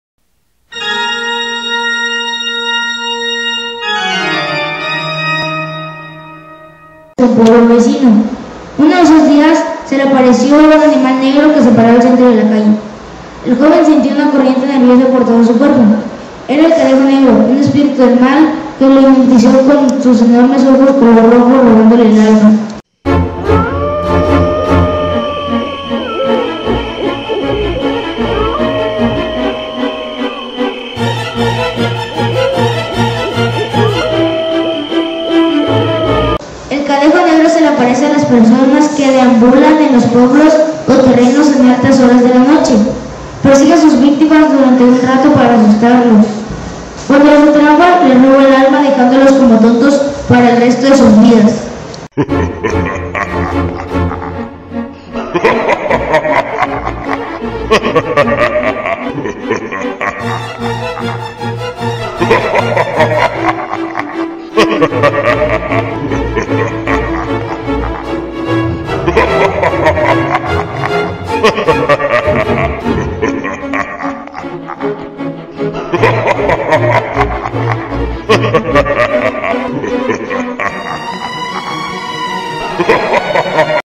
El Cadejo
Cortometraje realizado por los niños para el concurso a nivel Nacional de Casas Telmex